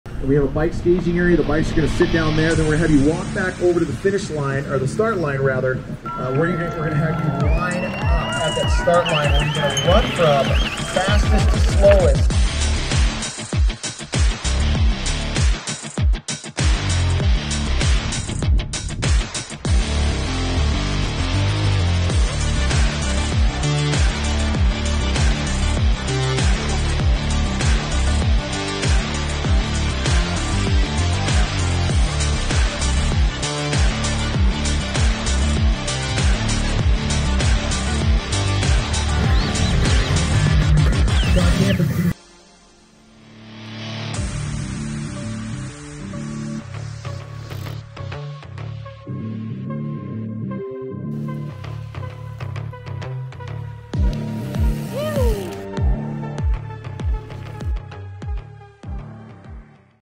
24-Hours in the Enchanted Forest
Hosted by Zia Rides, 24-Hours in the Enchanted Forest is a 12 or 24-hour endurance competition held on some fun singletrack in the Zuni Mountains. The course is a 13.5 mi. loop with a 700 ft. elevation gain and incorporates some technical rocky sections that are bound to make you smile. Wind and grind through meadows, pines, and aspens, or hang up your hammock and kick back with friends and family. Ride as a solo, duo, or team.

Race participants take over McGaffey campground, turning it into a temporary mountain biking village, with food trucks, music, and a party atmosphere, complete with movies and kid’s activities.